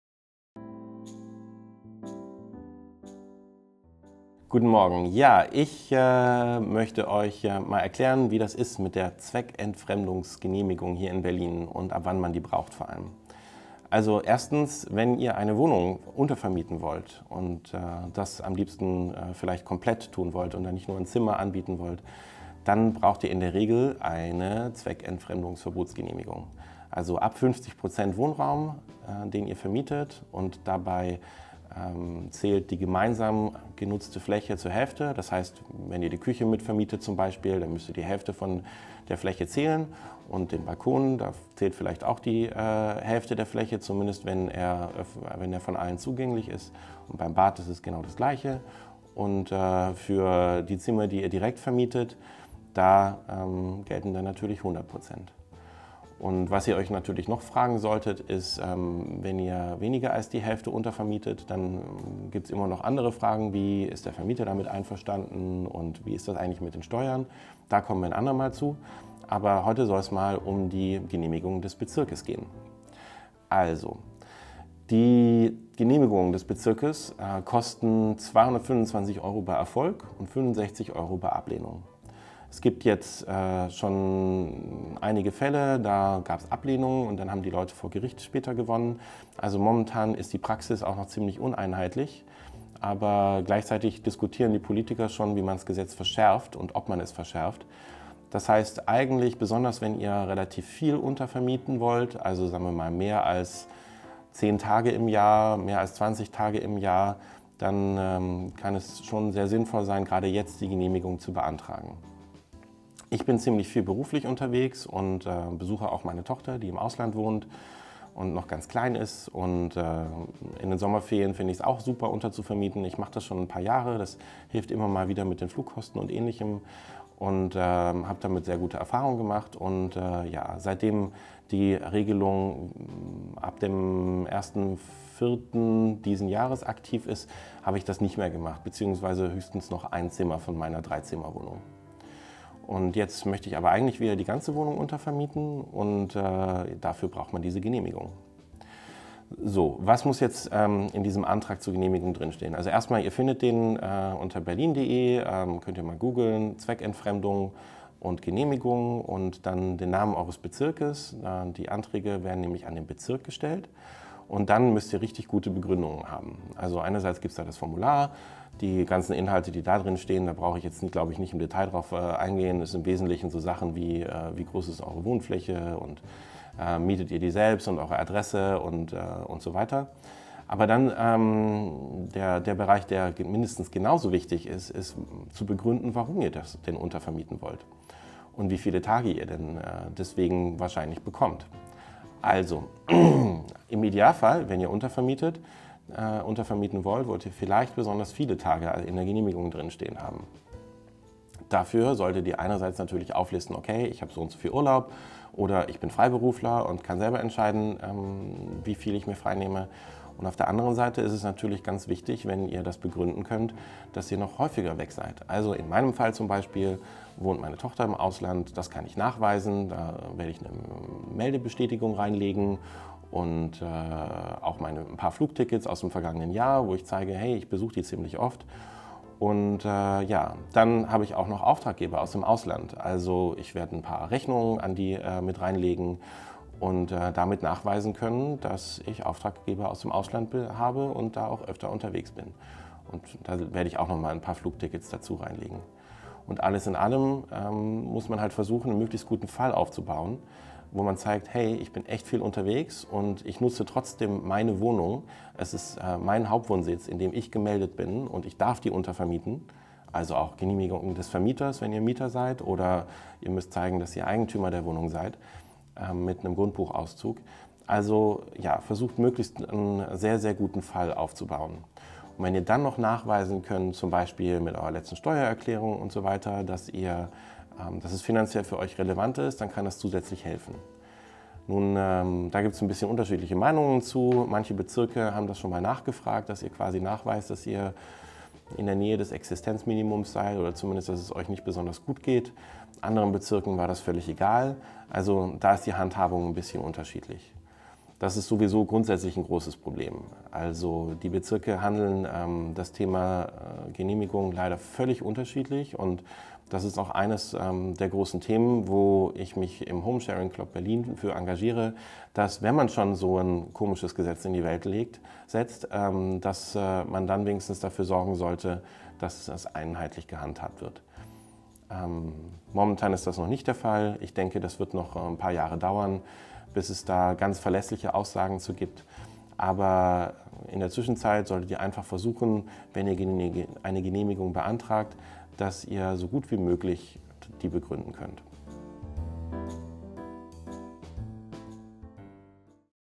Homesharing in Berlin: Zweckentfremdungsverbotsgesetz
Wann braucht ihr eigentlich eine Zweckentfremdungsgenehmigung als Homesharer und wie könnt ihr diese beantragen? Ich habe das für euch mal zusammengefasst.